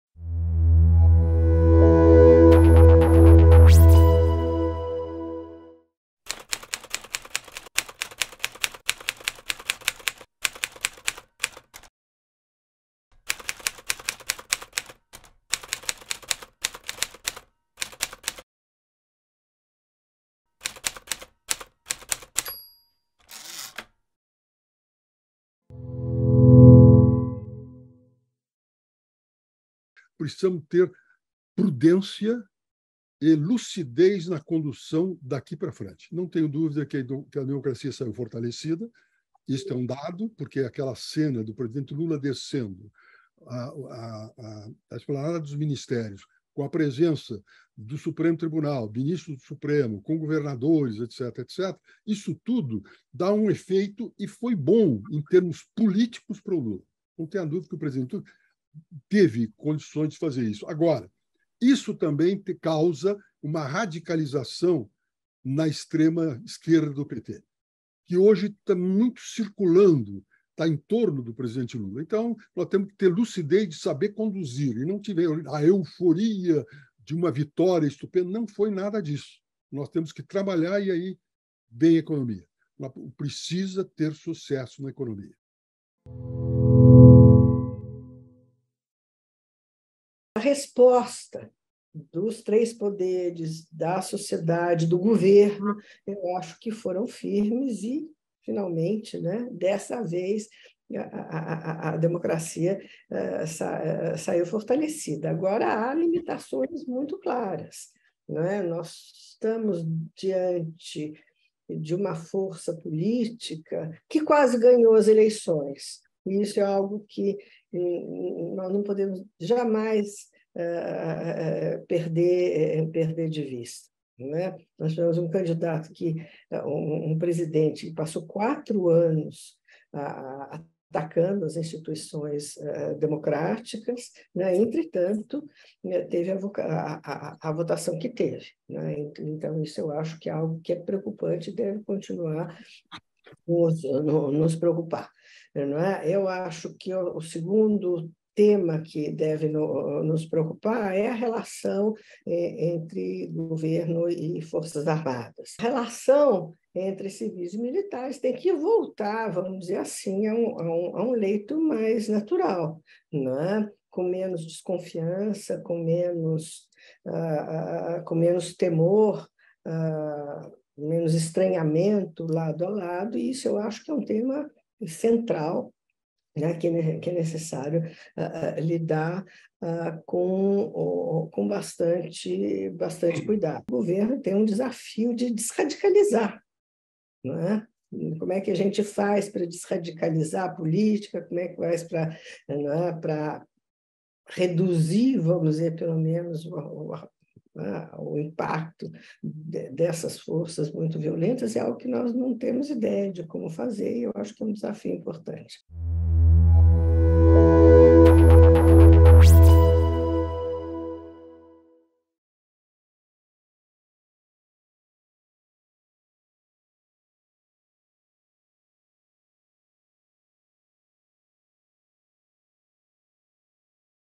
Resumo do webinar - Ataques de 8 de janeiro: o que aprendemos e como fortalecer a democracia?
A democracia sai fortalecida dos ataques golpistas de 8 de janeiro de 2023, mas o governo Lula precisa ampliar sua base de apoio no Congresso Nacional e na sociedade, trazendo a direita democrática e os setores mais liberais para mais perto. Também é fundamental buscar o entendimento correto sobre a questão militar e garantir que a economia brasileira tenha um bom desempenho nos próximos quatro anos.

Esta foi a conclusão do webinar “Atos antidemocráticos: Lições da História e Defesa da Democracia”, realizado recentemente pela Fundação FHC. Conversamos com Nelson Jobim, ex-ministro da Justiça, da Defesa e do STF, e Maria Hermínia Tavares de Almeida, cientista política e professora titular aposentada da Faculdade de Filosofia, Letras e Ciências Humanas (FFLCH-USP).

Este vídeo é um pequeno resumo do debate. O conteúdo completa está disponível no nosso